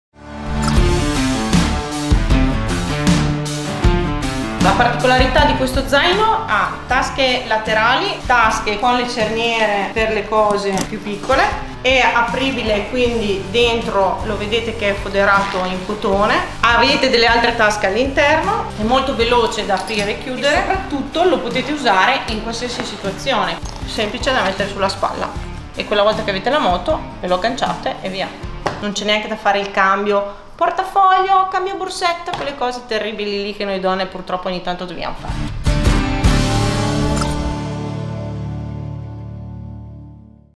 Canvas and leather backpack made by Wild Hog
Molto più di un semplice zaino, ma un compagno di viaggio.
Il nostro zaino unisce stile, qualità e utilità in un unico prodotto, adatto al quotidiano e per le avventure in sella della vostra moto.